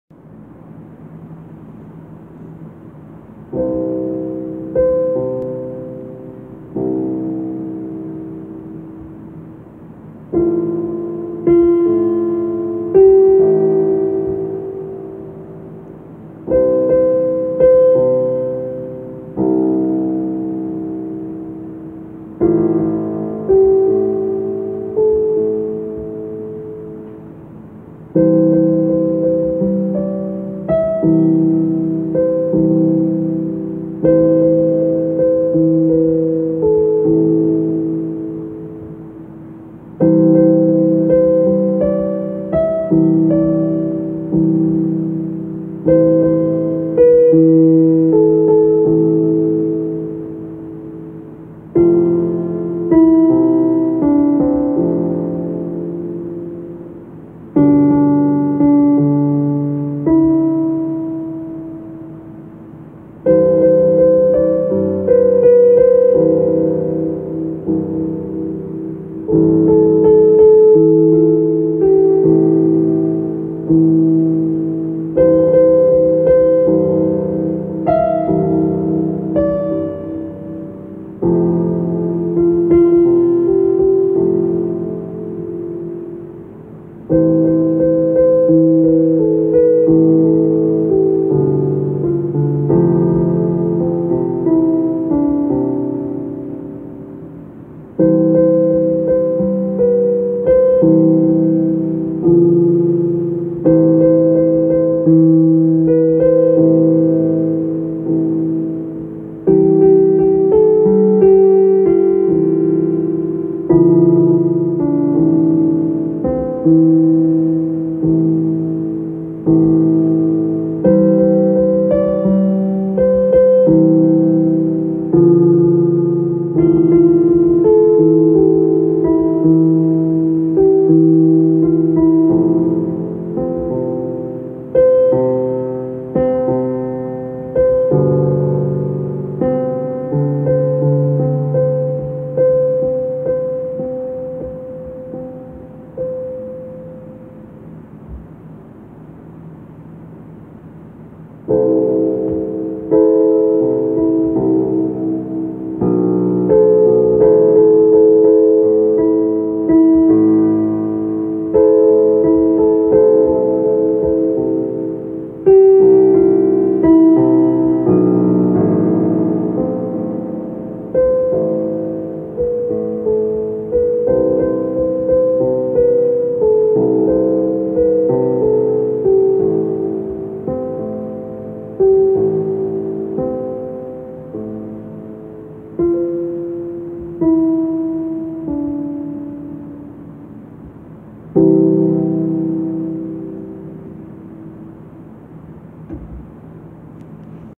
Twin flame intuitive soul song
As I sat down to play this intuitive song started coming through.
DF is saying where? where  are you?..where are you. I'm lonely and tired.
Dm is saying I don't know who I am. How can I? Where are you?

Divine collective says
You are all being purged and cleansed. This is symbolic of us receiving a new name and new clothes. You are being stripped of any and all things that no longer serve you. It is time to put on your new clothes and walk worthy of your calling. We are being corded together as the energetic fields collapse in and around us. We have seen our own blooms and know who and what we are. You have come to far to turn around now. Hold on to your knowledge  of who and what you are. The fire will be great but you are not alone in it. For something new to come the old has to go.